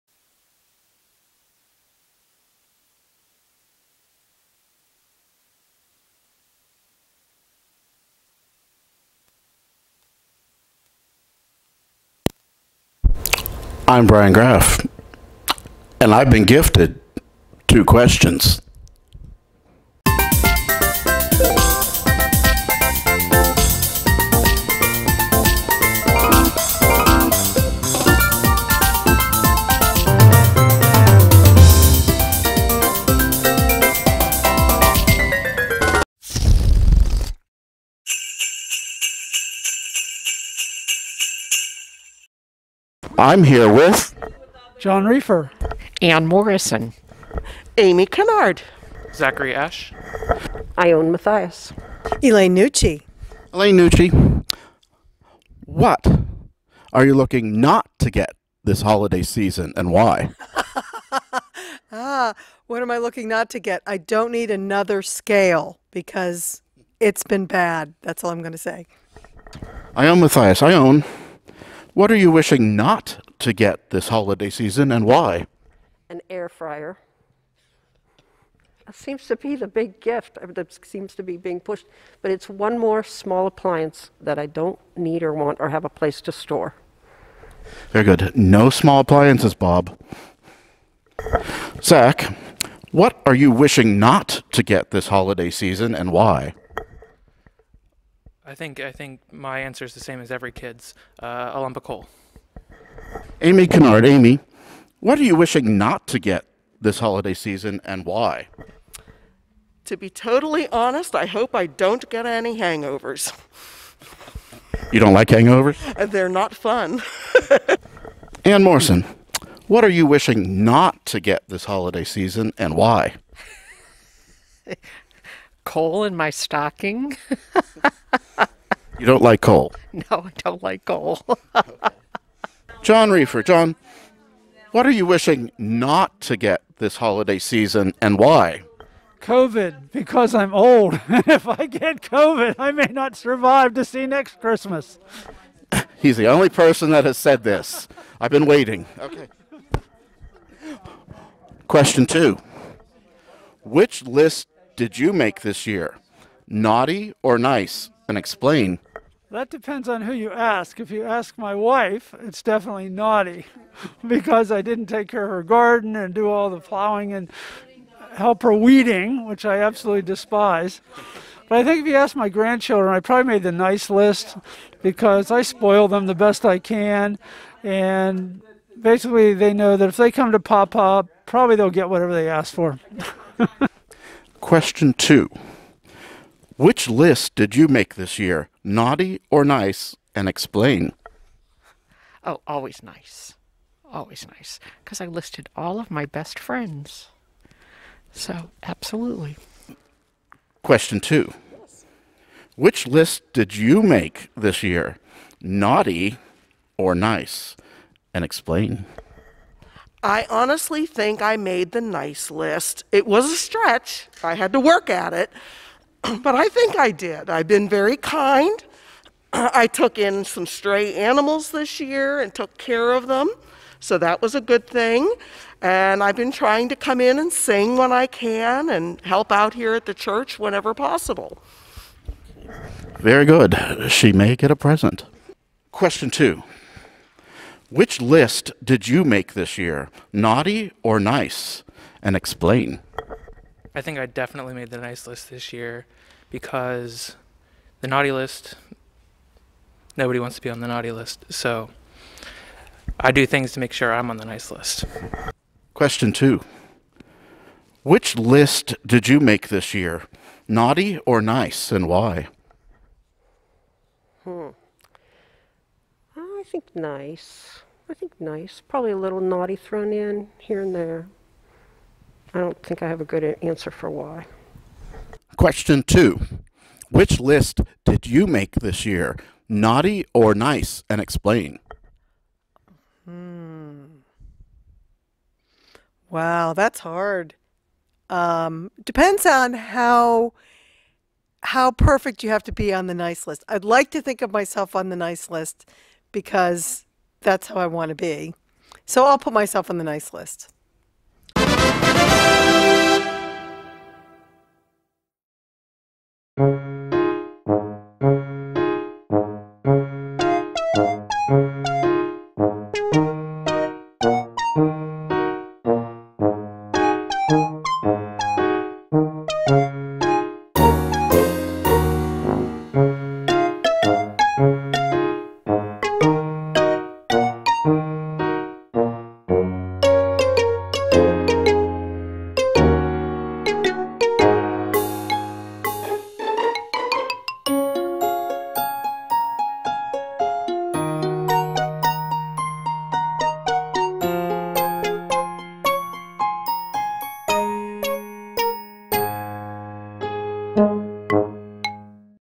"2 Questions?" December 26 2020
Episode 7: Christmas - Get to know our community with our fun interview show.